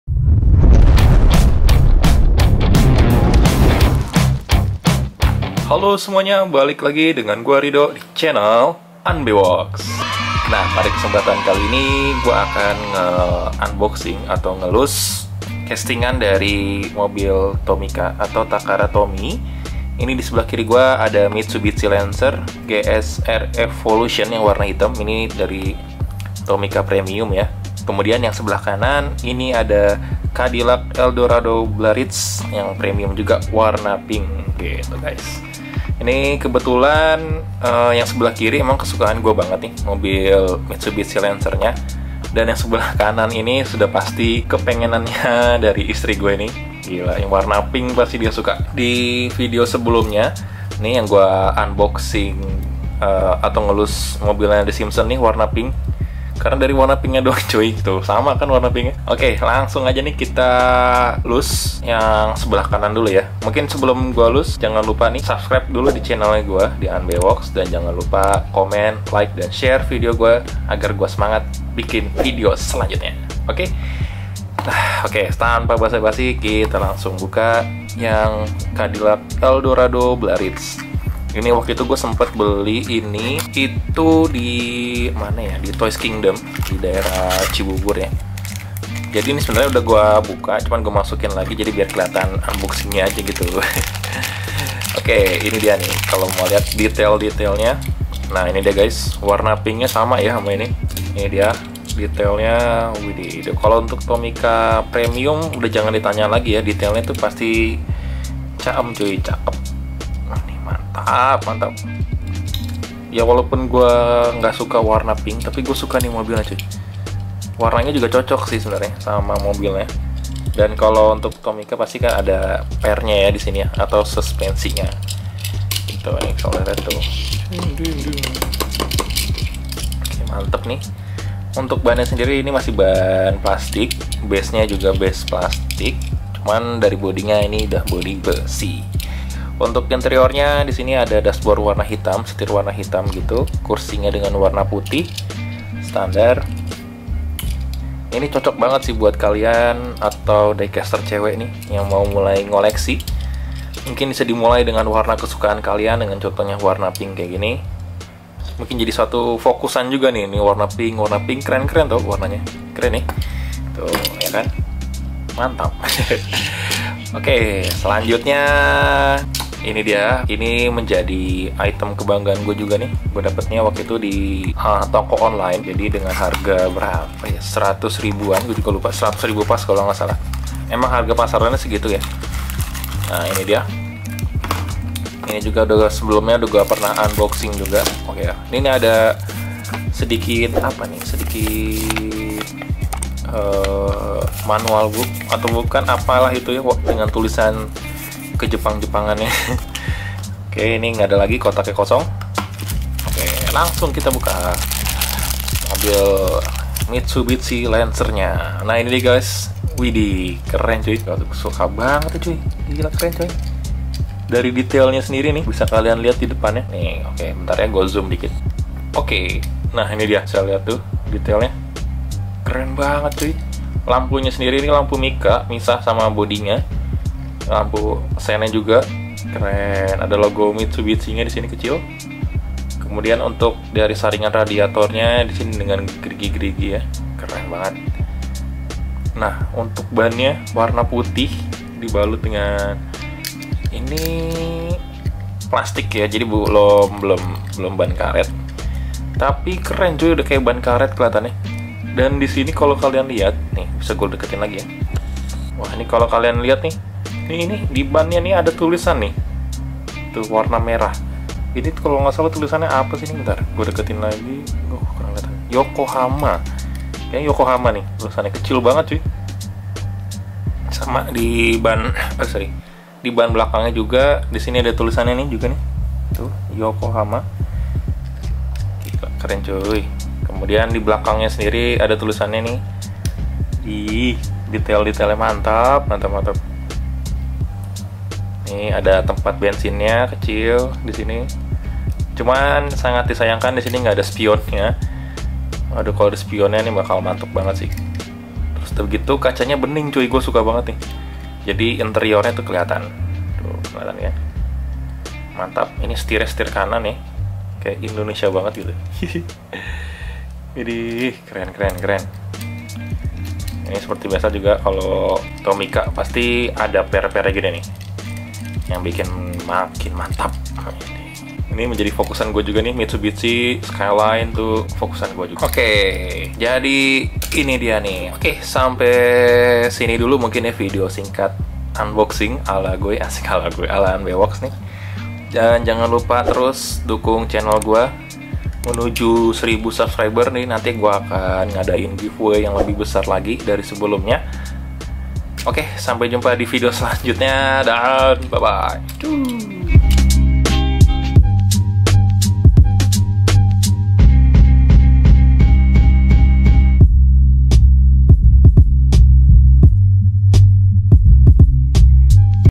UNBOXING TOMICA PREMIUM - DETAILNYA GOKIL SIH
Halooo semuanya, kali ini gue mau unboxing lagi nih gaes, seri tomica premium pilihan gue dan istri gue hahahaha. Yuk simak videonya sampe habis...